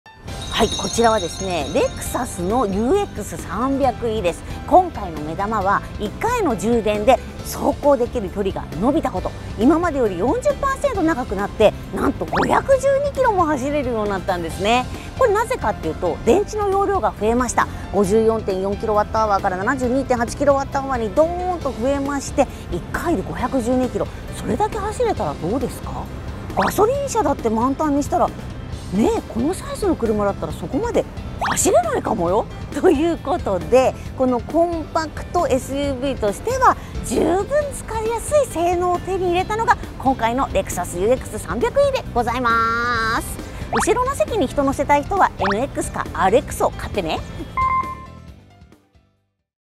竹岡 圭のレクサスUX300e試乗【TAKEOKA KEI & LEXUS UX300e 】
@圭Tube☆竹岡圭のカートークバラエティ☆